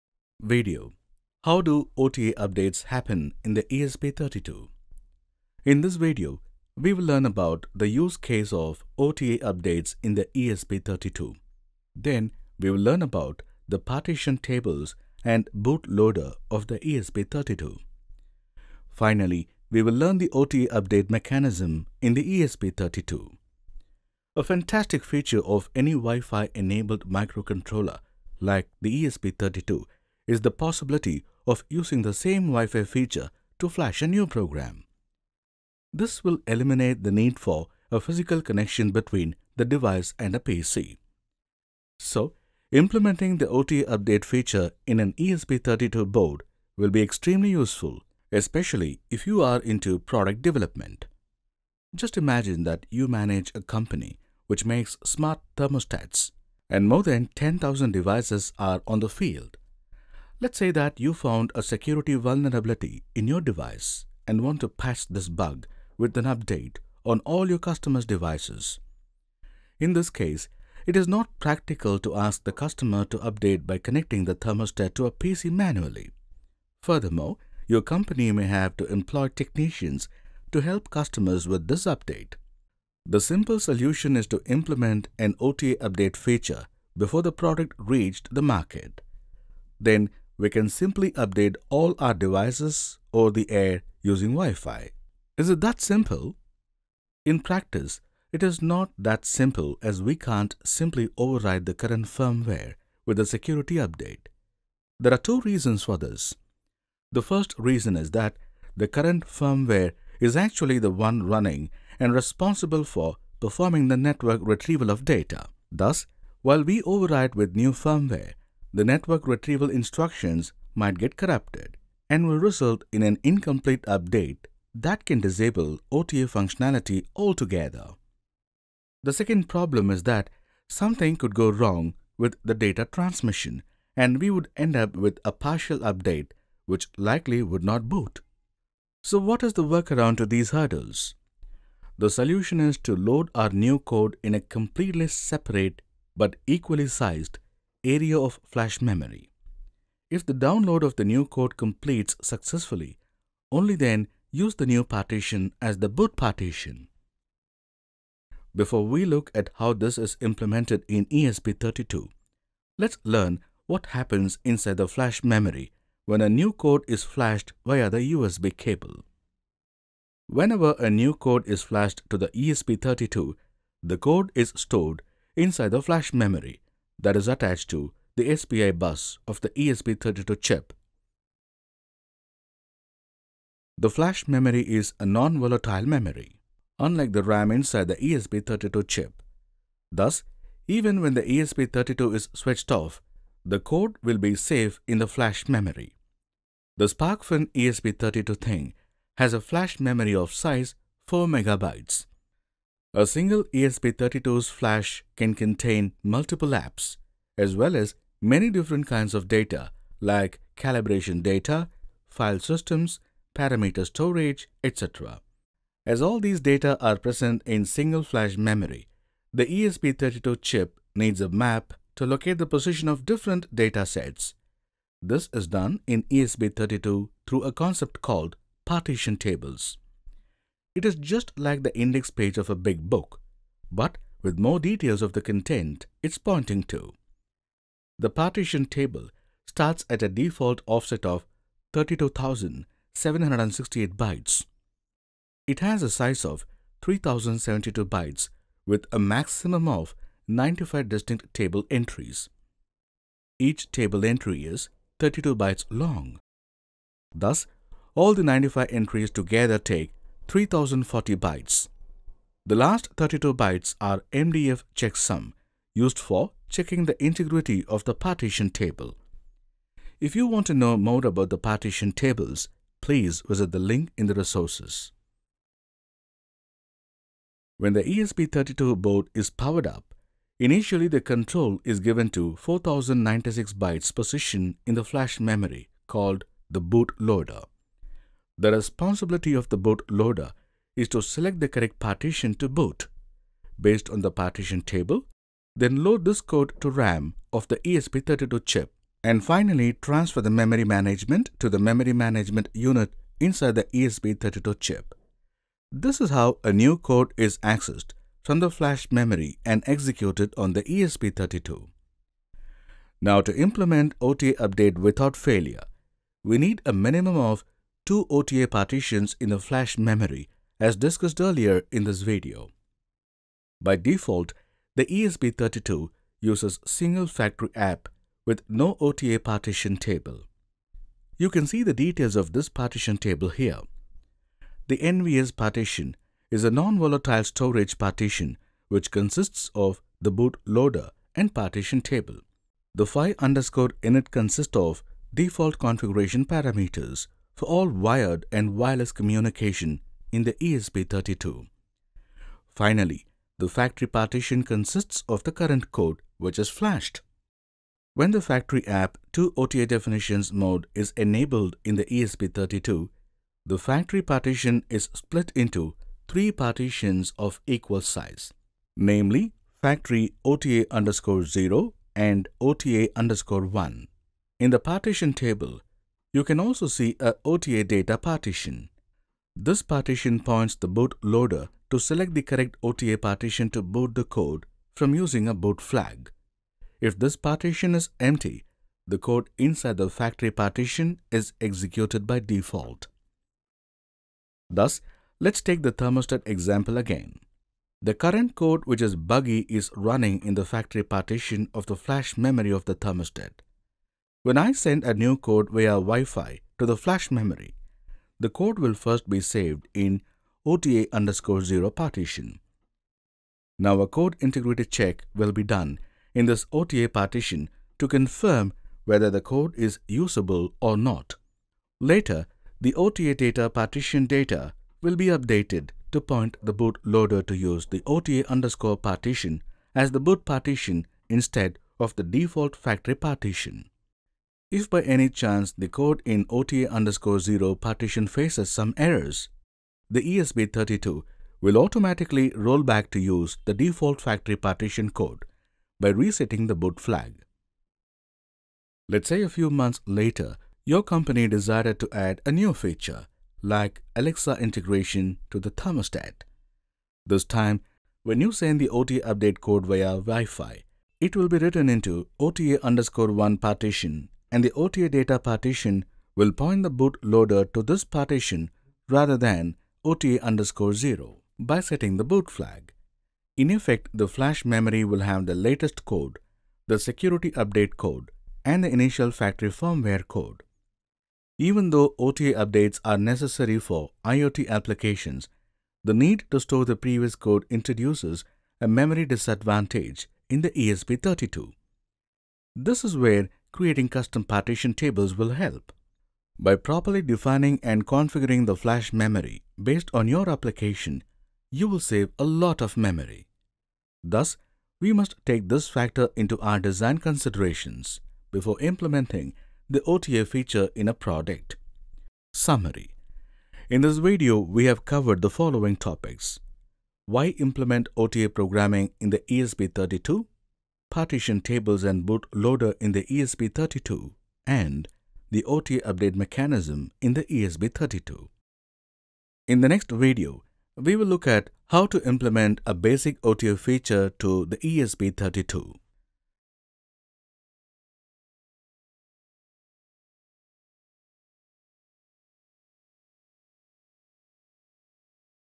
HOW DO OTA UPDATES HAPPEN IN THE ESP32? | ADVANCED ESP32
How do OTA Updates happen in the ESP32?
In this video, we will learn about the use case of OTA Updates in the ESP32. Then we will learn about the Partition Tables and Bootloader of the ESP32. Finally, we will learn the OTA update mechanism in the ESP32.

(If you liked this video and would like to buy our course, you are in for a pleasant surprise. Original price is $195.

 A fantastic feature of any WiFi-enabled microcontroller like the ESP32 is the possibility of using the same WiFi Feature to flash a new program.This will eliminate the need for a physical connection between the device and a PC. So, implementing the OTA Update feature in an ESP32 board will be extremely useful especially if you are into product development.

 Just imagine that you manage a company which makes smart thermostats and more than 10000 devices are on the field. Let's say that you found a security vulnerability in your device and want to patch this bug with an update on all your customer’s devices. In this case, it is not practical to ask the customer to manually update by connecting the thermostat to a PC. Furthermore, your company may have to employ technicians to help customers with this update.

 The simple solution is to implement an OTA Update feature before the product reached the market. Then we can simply update all our devices over the air using WiFi. Is it that simple?
 In practice, it is not that simple as we can’t simply overwrite the current firmware with the security update. There are two reasons for this.

 The first reason is that the current firmware is actually the one running and responsible for performing the network retrieval of data. Thus, while we overwrite with new firmware, the network retrieval instructions might get corrupted and will result in an incomplete update that can disable OTA functionality altogether.

 The second problem is that something could go wrong with the data transmission and we would end up with a partial update which likely would not boot. So, what is the workaround to these hurdles?

 The solution is to load our new code in a completely separate, but equally sized, area of flash memory. If the download of the new code completes successfully, only then use the new partition as the boot partition.

Before, we look at how this is implemented in ESP32, let's learn what happens inside the flash memory when a new code is flashed via the USB Cable.

 Whenever a new code is flashed to the ESP32, the code is stored inside the flash memory that is attached to the SPI Bus of the ESP32 chip. The flash memory is a non-volatile memory, unlike the RAM inside the ESP32 Chip. Thus, even when the ESP32 is switched off, the code will be safe in the Flash Memory. The Sparkfun ESP32 Thing has a flash memory of size 4 MegaBytes.

 A single ESP32’s flash can contain multiple apps, as well as many different kinds of data like calibration data, file systems, parameter storage, etc. As all these data are present in single flash memory, the ESP32 chip needs a map to locate the position of different data sets. This is done in ESP32 through a concept called Partition Tables. It is just like the Index page of a big book, but with more details of the content, it’s pointing to. The partition table starts at a default offset of 32768 Bytes. It has a size of 3072 bytes with a maximum of 95 distinct table entries. Each table entry is 32 bytes long, thus all the 95 entries together takes 3040 bytes. The last 32 bytes are MDF Checksum used for checking the integrity of the partition table. If you want to know more about the partition tables please visit the link in the resources.

 Summary
In this video, we have covered the following topics
●     Why implement OTA Programming in the ESP32
●     Partition Tables and Bootloader in the ESP32
●     The OTA Update Mechanism in the ESP32